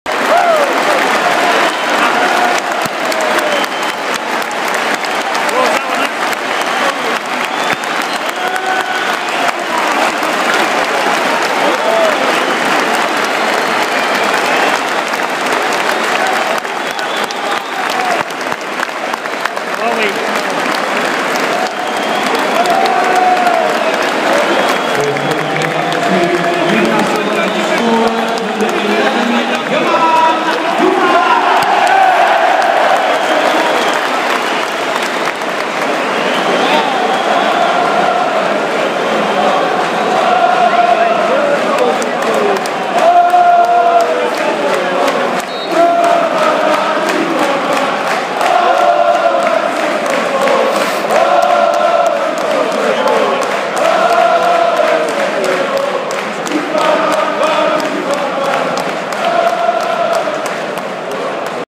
Newcastle fans, Gouffran song
Newcastle fans after Gouffran goal vs Southampton